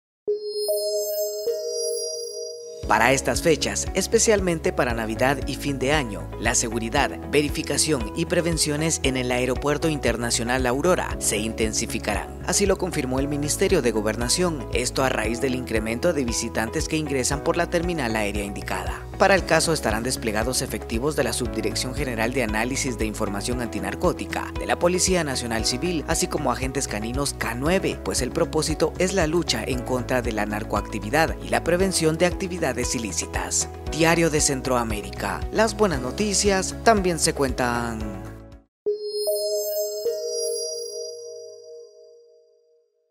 Seguridad incrementará en el AILA
Mingob desplegará personal y agentes caninos K9.